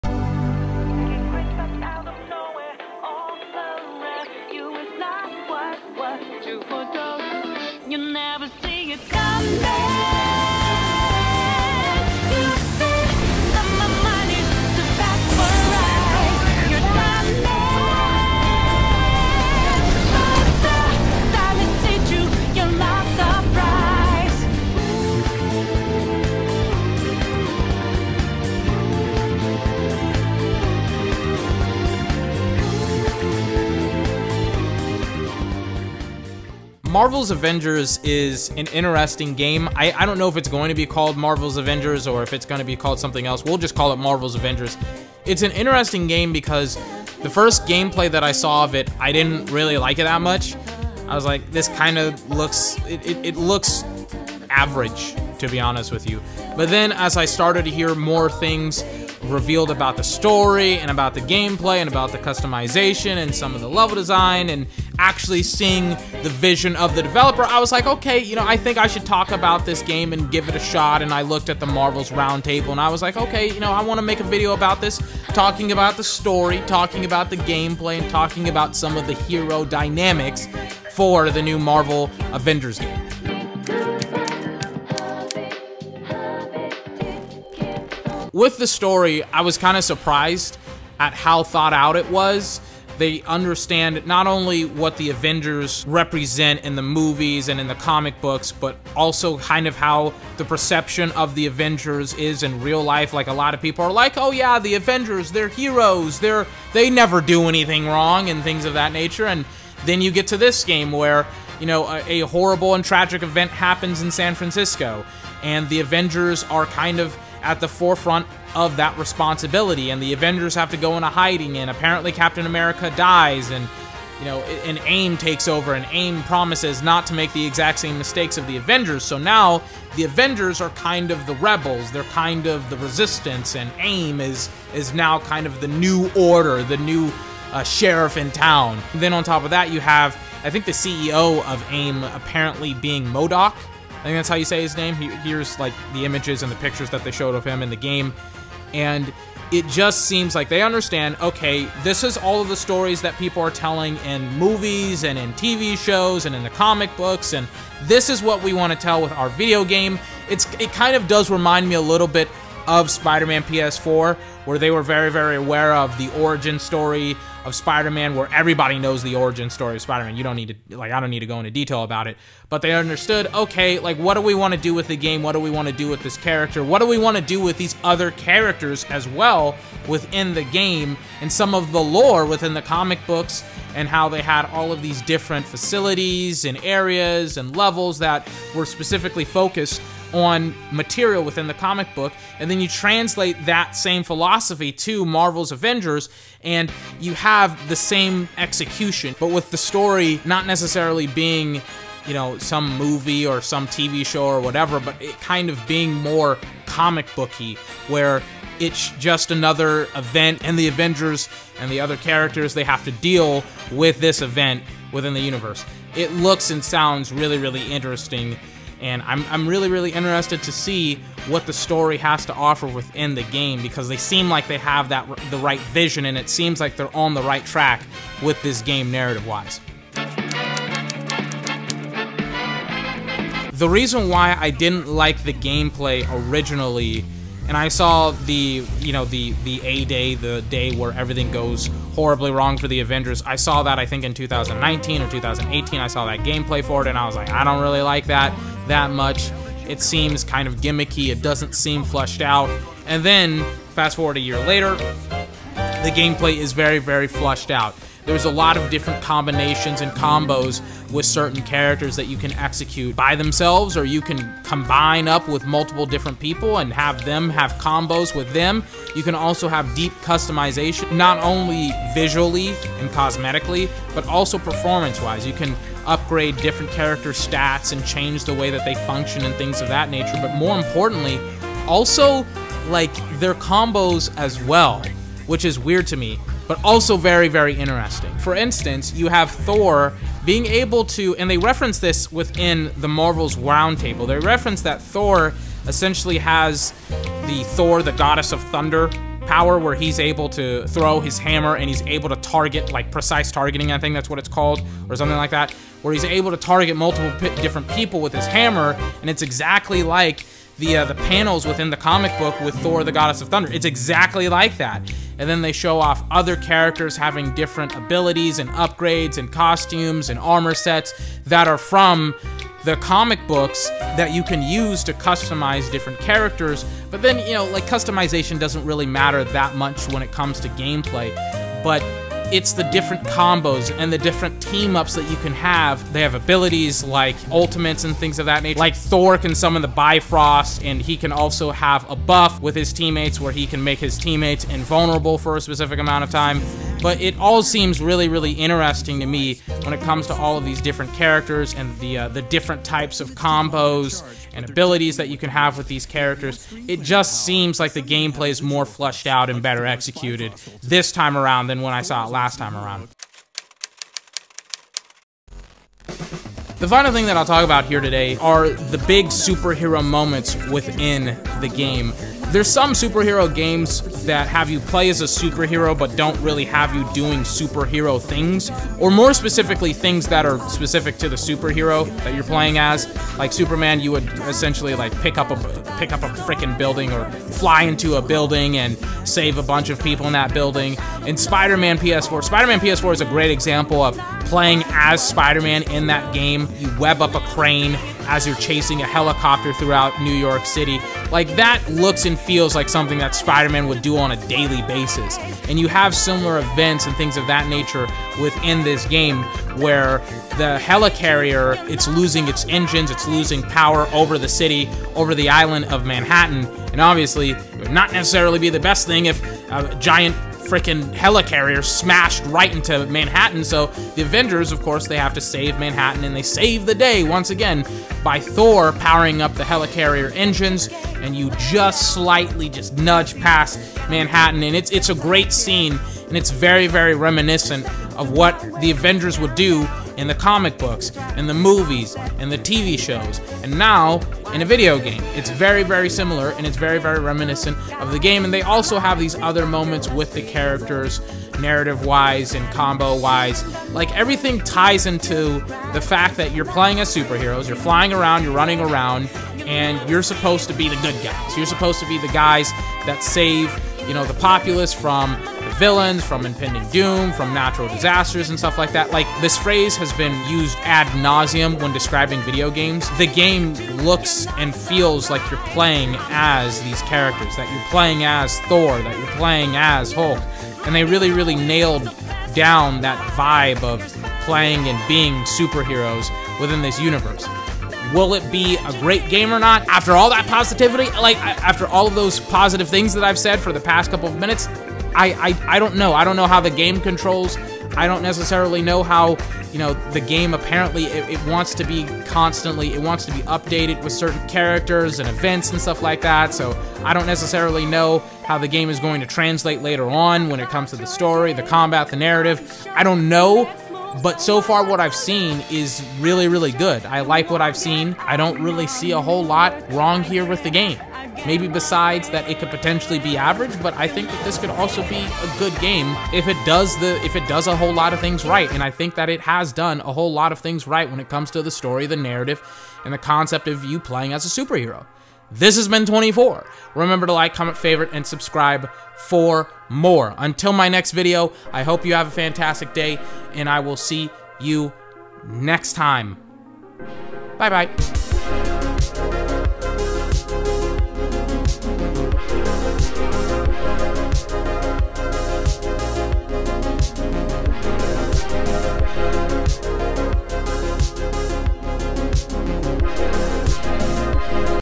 A Video Game Analysis: Marvel's Avengers.
Today I'll take a look at the new upcoming game from Crystal Dynamics, Marvel's Avengers. I'll look at the gameplay, story, and the hero dynamics within the game.